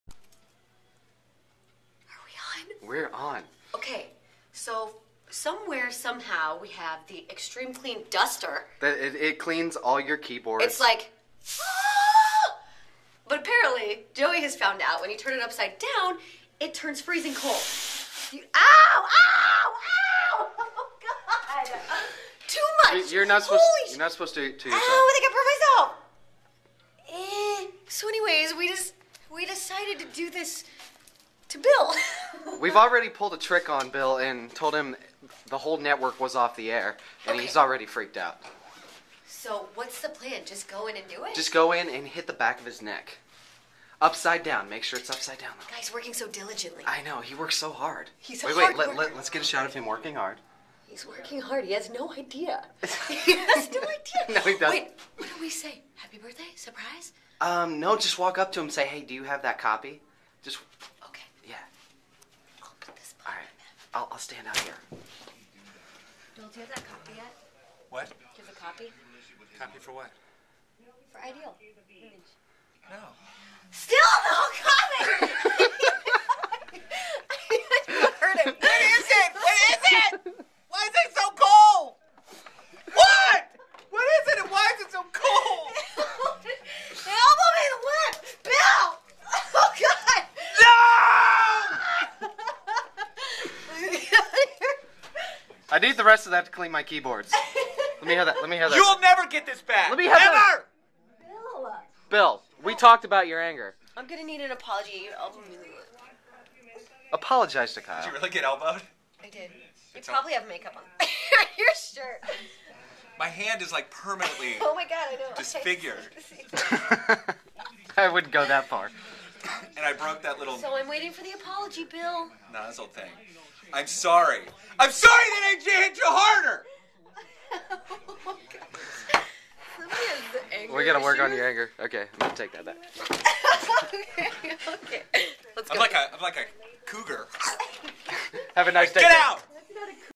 DUSTER BURN!
Joey and Kyle play a prank on Bill, using duster!